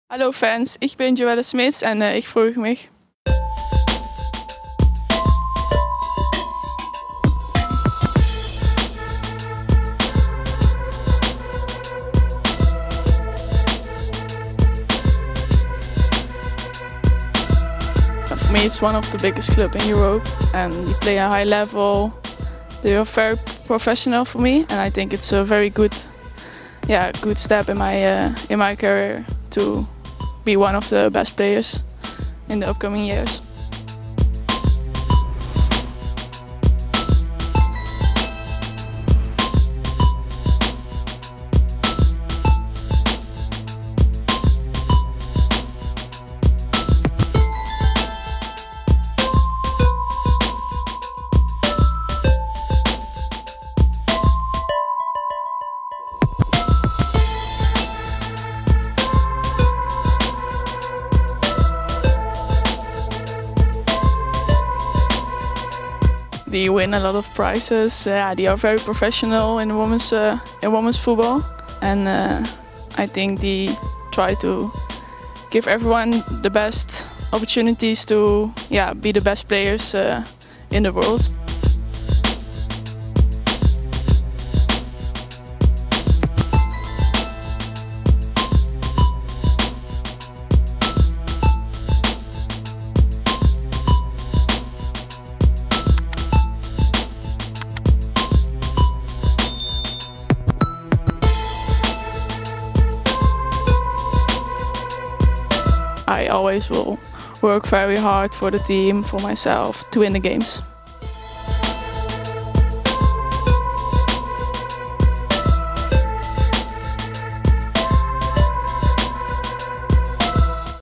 Wölfinnen verpflichten Joëlle Smits, die niederländische Sturmhoffnung!
Die VfL Wolfsburg Frauen verpflichten Joëlle Smits von der PSV Eindhoven aus den Niederlanden! Die erst 20-jährige Stürmerin kommt mit der Empfehlung von bisher 61 Toren in 57 Spielen für PSV und Twente Enschede in der Eredivisie Vrouwen zu den Wölfinnen. Verfolgt die Nationalspielerin in ihren ersten Momenten auf dem Wölfe-Gelände und in ihrem ersten Interview, in dem sie die Entscheidung für Wolfsburg und ihre ambitionierten Ziele erklärt! Was sagt ihr zum Neuzugang?